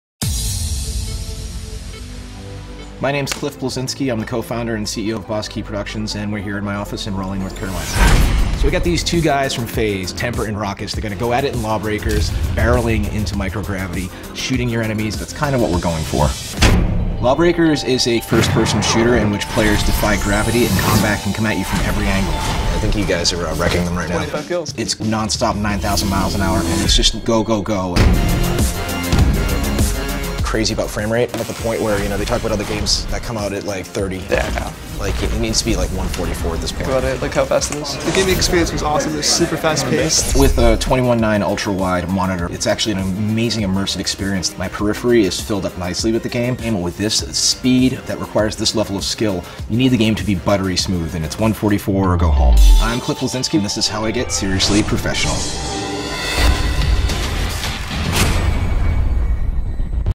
LG Gaming Monitors...Seriously Professional!
LG'S GAMING MONITORS: GET SERIOUSLY PROFESSIONAL IN YOUR GAMING!

Watch FaZe Rawkus and FaZe Temperrr meet Cliff Bliszinksky, creator of LawBreakers, as they prepare for their upcoming battle on LG 21:9 UltraWide® monitors. Get the advantage that the pro's use—stop into any Video & Audio Center today as ask to see our complete line of LG Gaming Monitors.